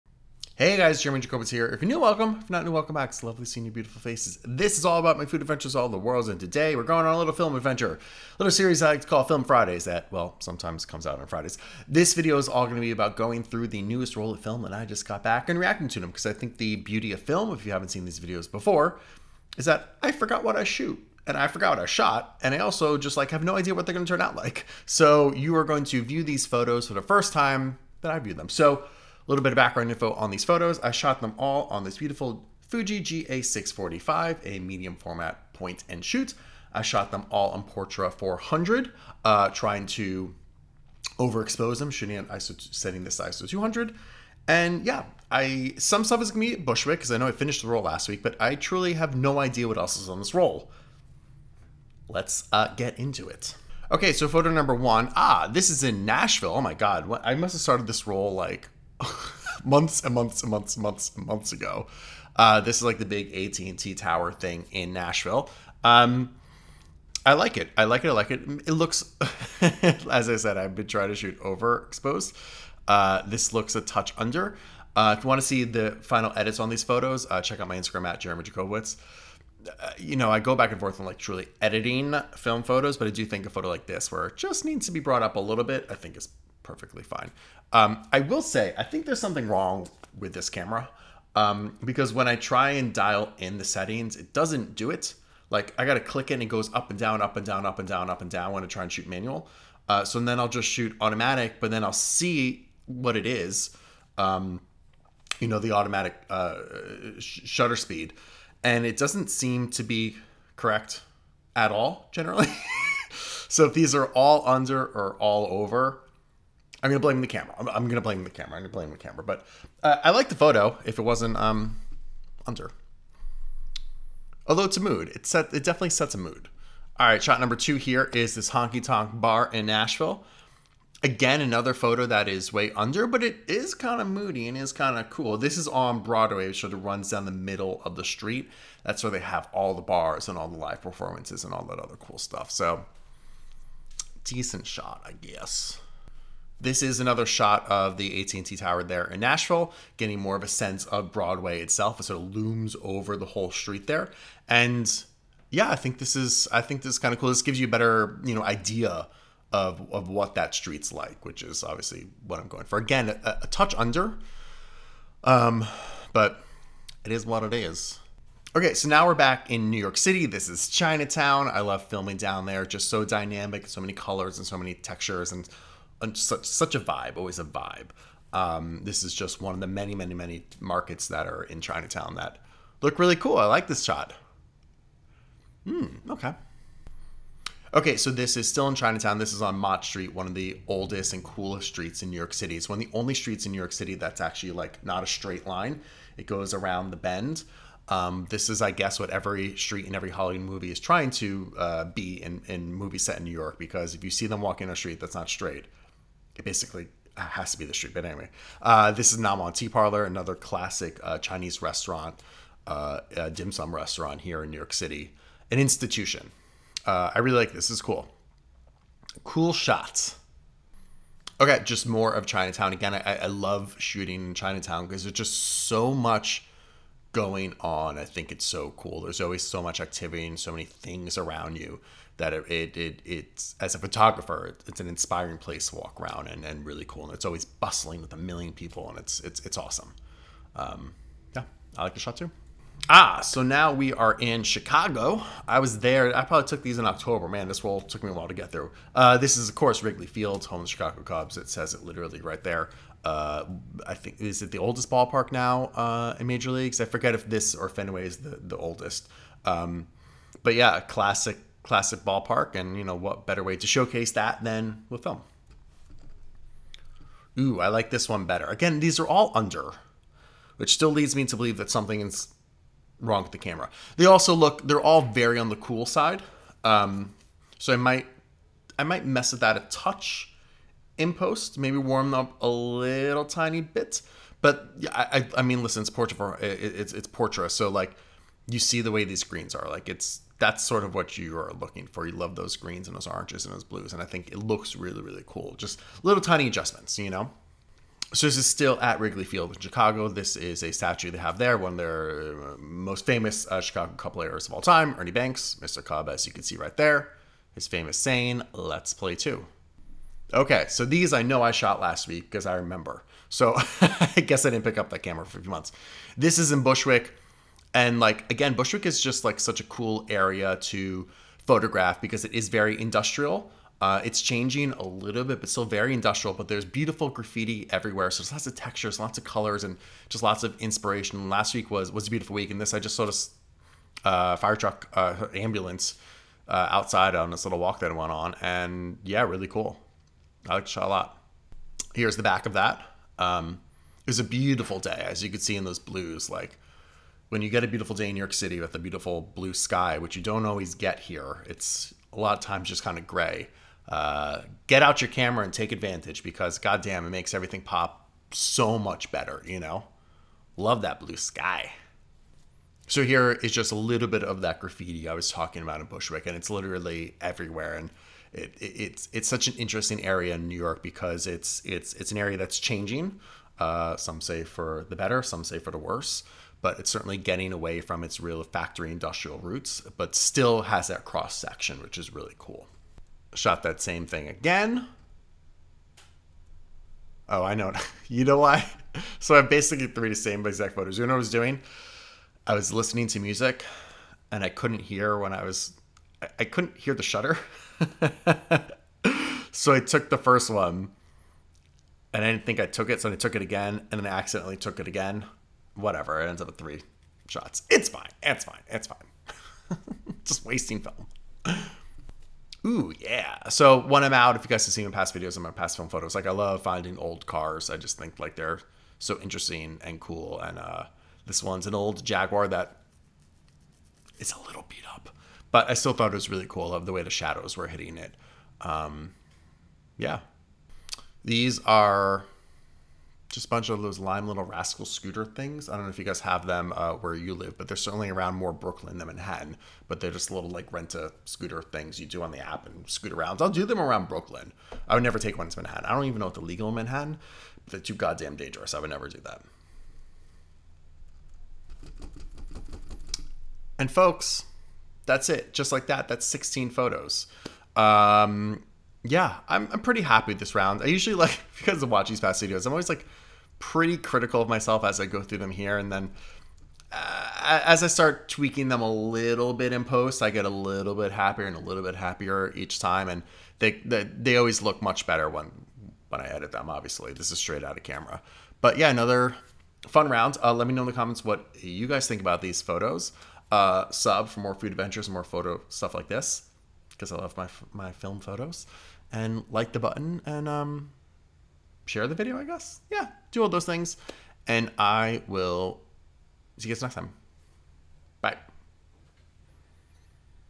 Looking At My Film Photos! Portra 400 on Fuji GA645 | Jeremy Jacobowitz
Jeremy Jacobowitz looks through his film photos he took on his Fuji GA645 medium format point and shoot with a roll of portra 400 film.   Looking back at photos he took in Nashville, Chicago, and New York City